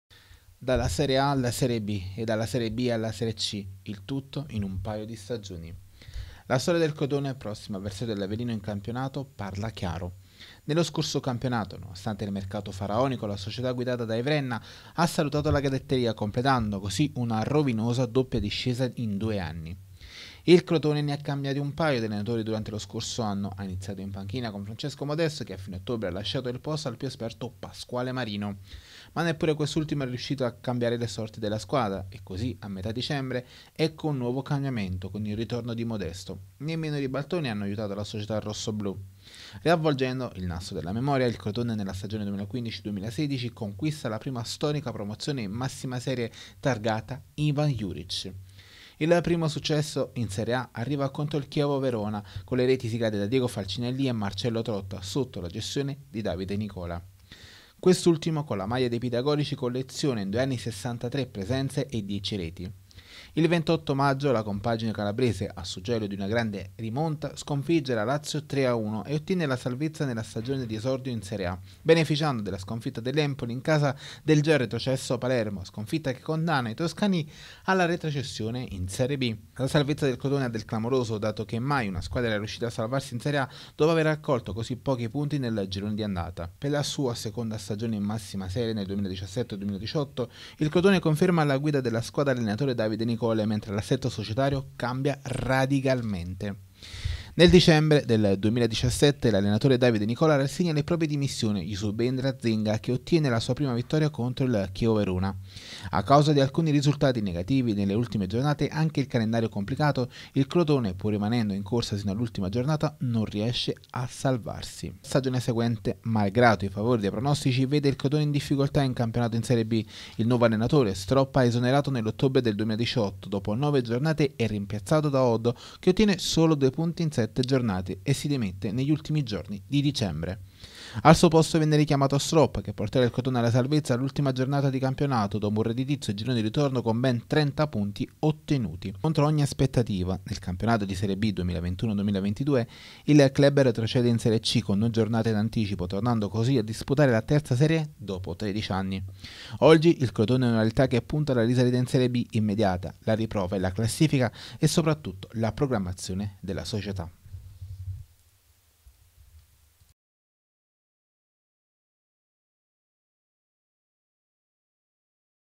Crotone, due anni da dimenticare: dalla Serie A alla LegaPro
Dopo  13 anni la compagine rossoblu si ritrova a lottare nel campionato di terza divisione, cancellando così le due storiche promozione in massima serie